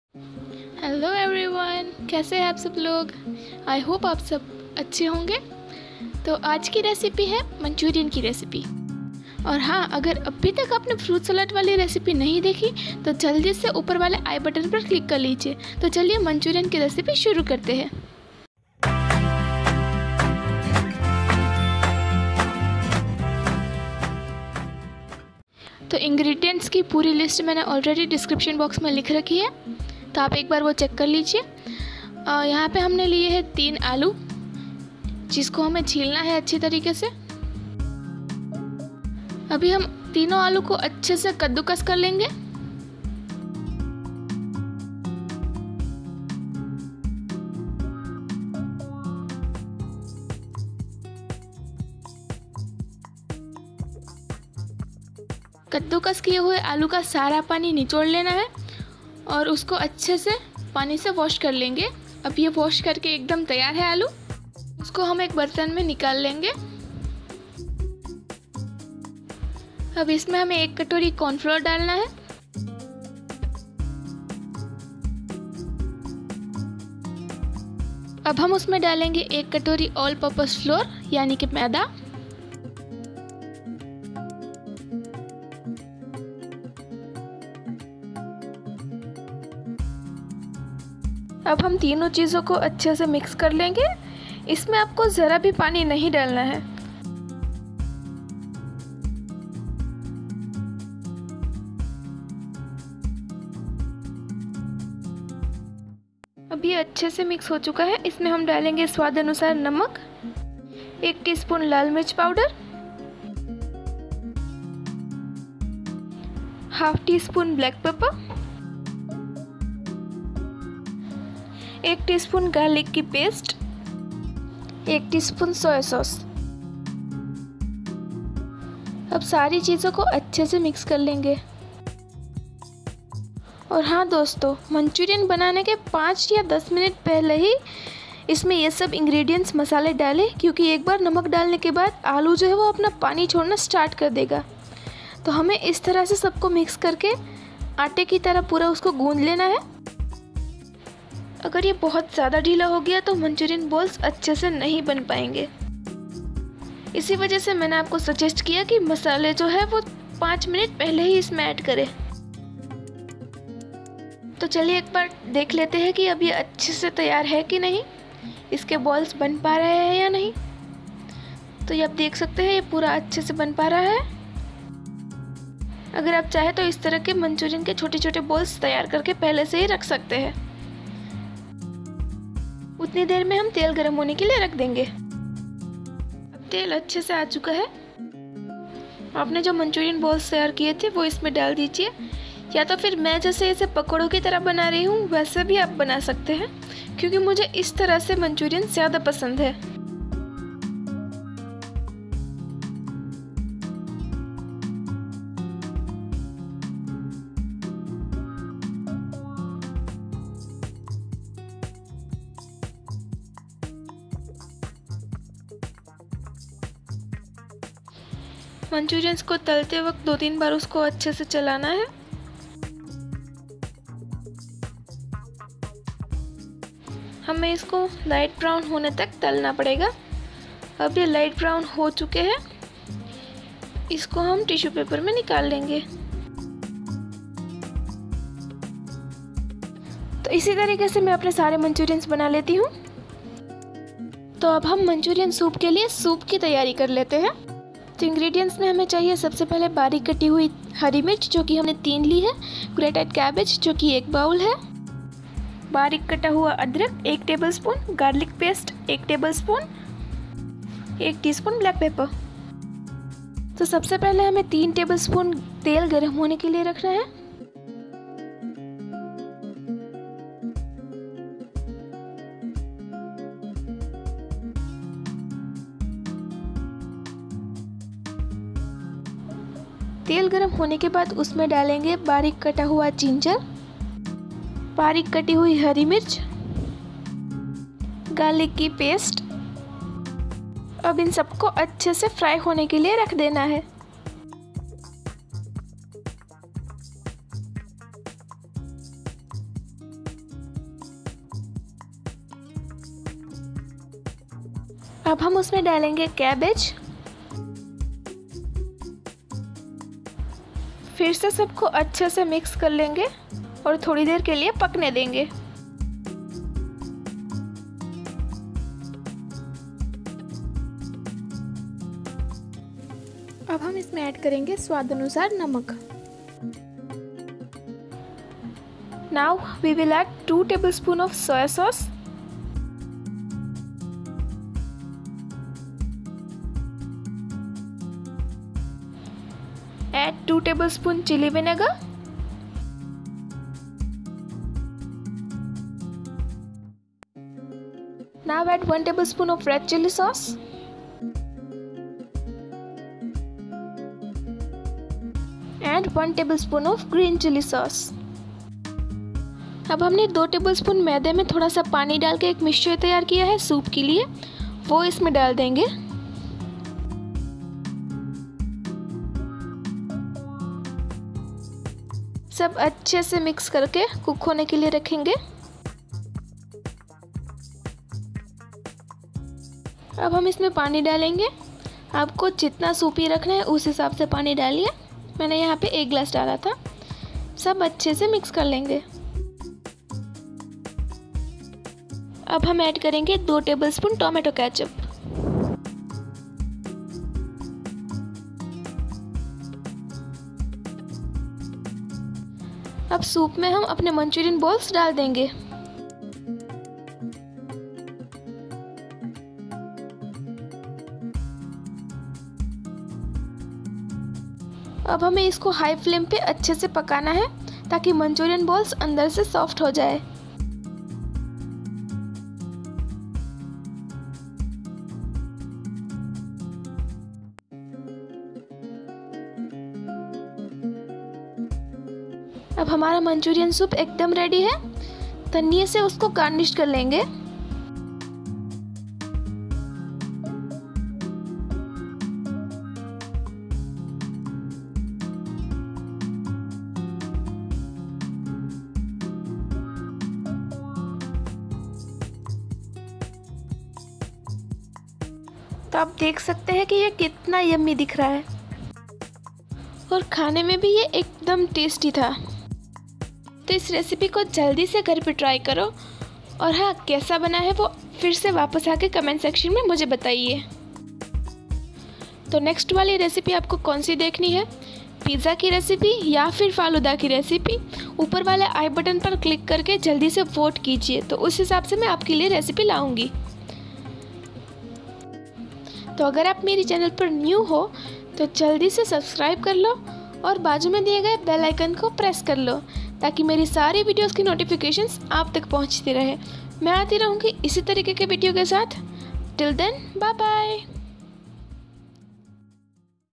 MANCHURIAN SOUP / GRAVY RECIPE || SHREEJI KITCHEN
Hello you all... How are you guys.?

I hope you all are fine . I am here with my new recipe. " MANCHURIAN GRAVY RECIPE "

Here are the ingredients:

For Manchurian

Potato - 300gm (3 big piece)

Corn flour - 1 bowl

All purpose flour (maida) - 1 bowl

Chilli powder - 1 ts

Black paper - 1 ts

For Gravy

Chopped Ginger - 1tbsp

Chopped green chilli - 3 tbsp

Chopped cabbage - 1 bowl

Green chilli sauce - 1 tbsp

I hope  this recipe will help you out in one or other way.